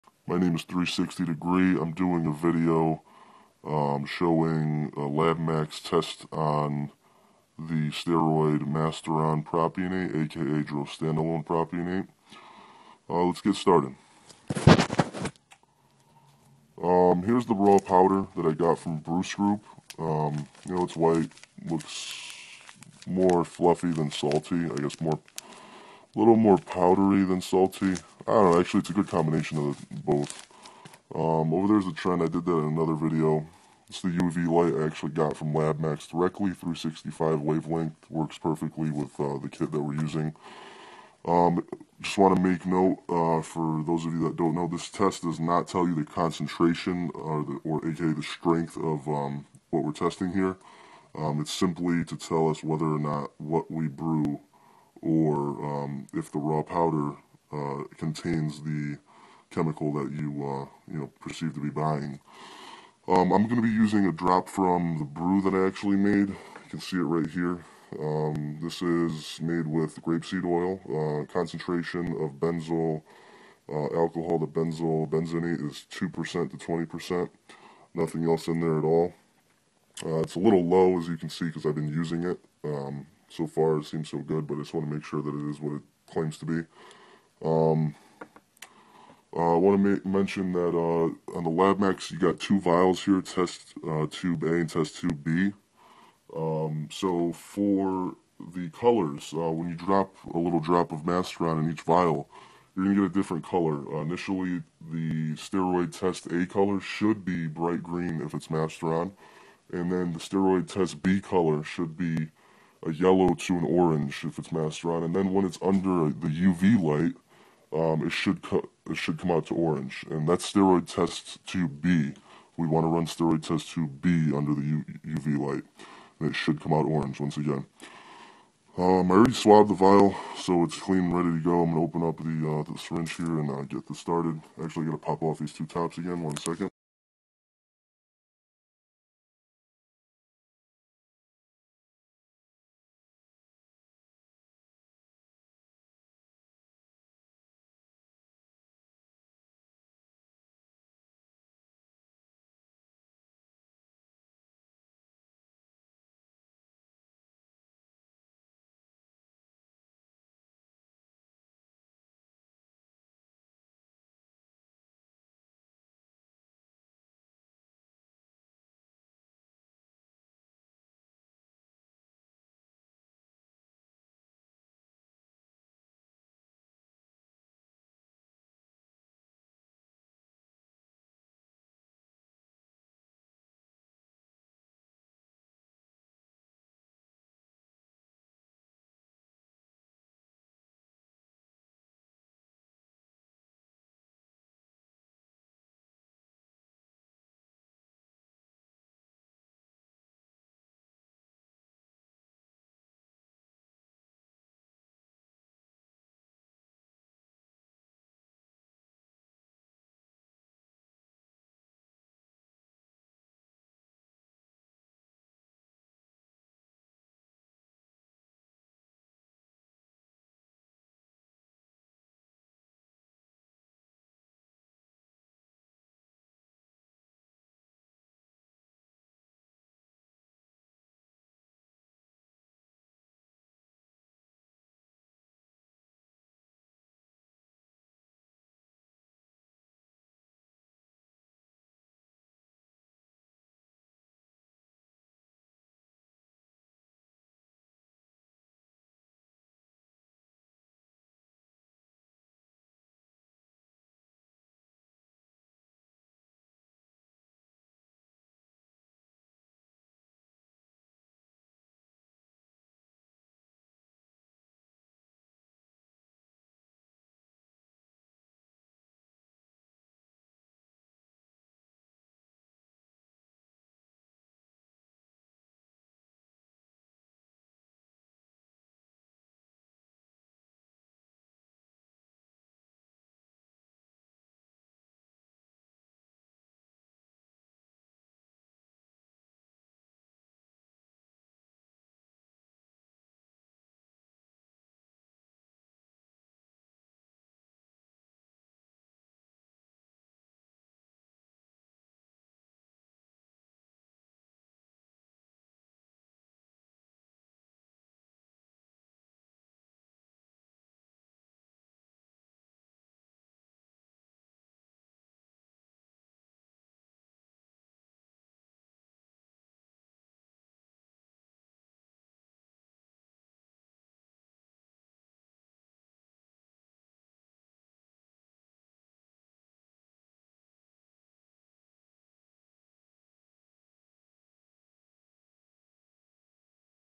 Drostanalone Propionate Labmax Test
In this video I used a Labmax.ca test kit on a Drostanalone Propinate oil (aka Masteron Propionate) that was brewed using raw powder from BruceGroup. The Labmax kit tells us whether or not the test sample contains the correct chemical, in this case Drostanalone Propionate. **This test does not tell us the concentration of the Drostanalone or how pure it is.

According to the Labmax website, the test sample contains Drostanalone Propionate IF: Under no UV light sample tube A turns bright green, and sample tube B turns yellow to orange, and then Under UV light sample tube B turns orange.

In my observation this sample did not pass the test because the sample must meet all 3 color requirements according to Labmax and in this case it did not. The Labmax website indicated that sample A should turn bright green under no UV light, yet this turned rusty brown/deep orange color.

Sample B under no UV light did match the Labmax website color, turning a yellow to orange color. This turned a deep orange color which I believe is acceptable because that is techinically orange.

Finally under UV light, sample B turned orange, again a bit on the darker side but still orange.

According to the Labmax website, if a chemical is mixed with another chemical it will throw the test off and show different colors. That's one possibility here. The other possibility is that this contains another product entirely.